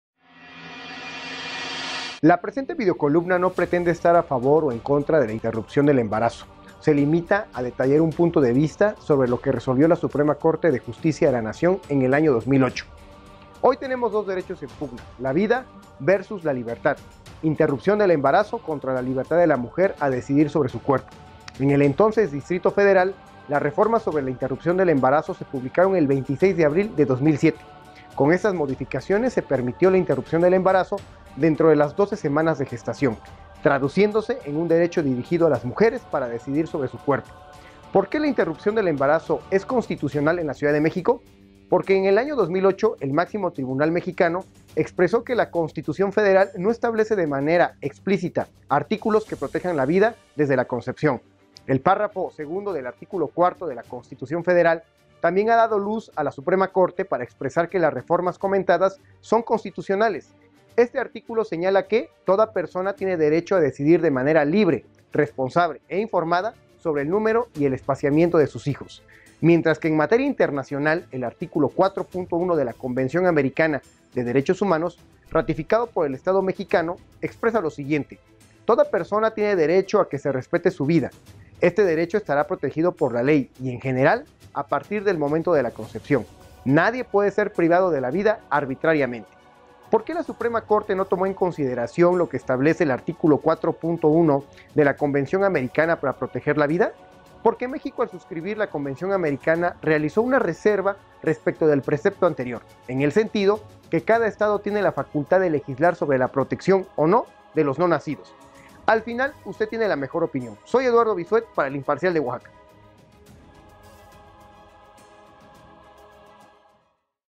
Videocolumna: Interrupción del embarazo vs. Constitución
¿Por qué la interrupción del embarazo es Constitucional en la CDMX? Porque en el año 2008 se expresó que la Constitución Federal no establece de manera explícita artículos que protejan la vida desde la concepción.
La Opinion de Eduardo Bizuet